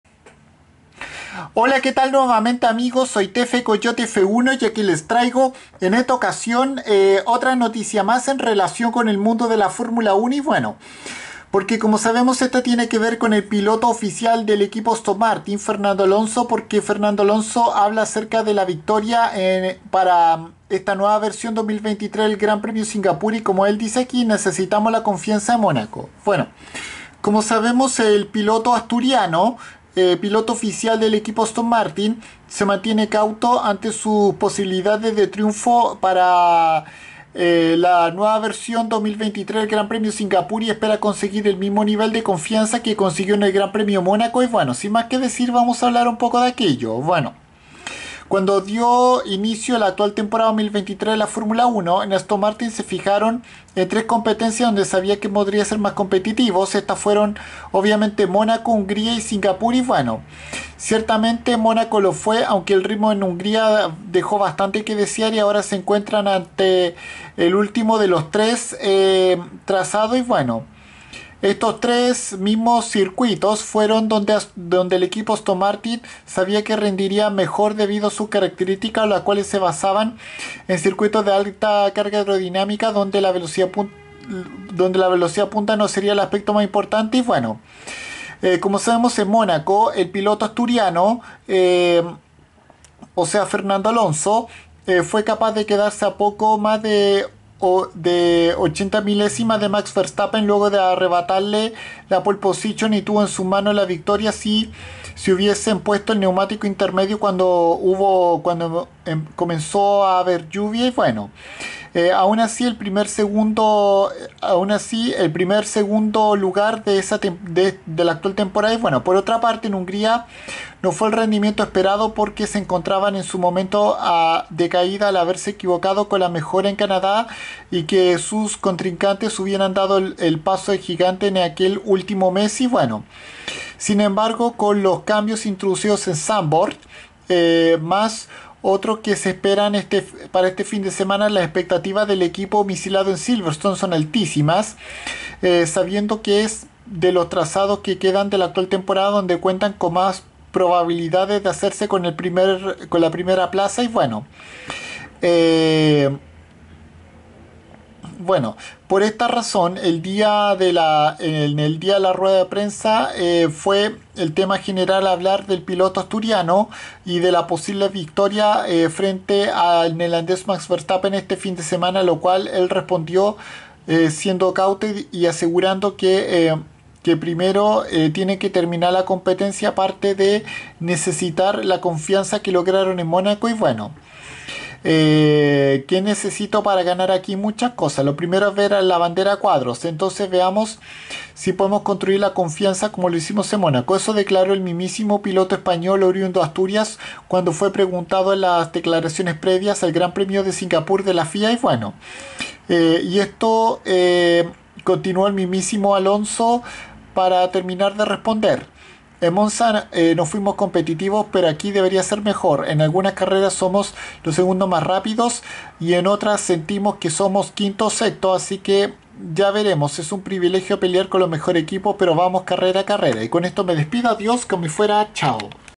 Fernando Alonso habla acerca del triunfo en el GP de Singapur
El piloto español oriundo de Asturias actual piloto oficial de Aston Martin F1 Team es bastante cauto acerca de su probabilidad de victoria en Marina Bay y espera tener confianza como la que tuvo en el reciente Gran Premio de Mónaco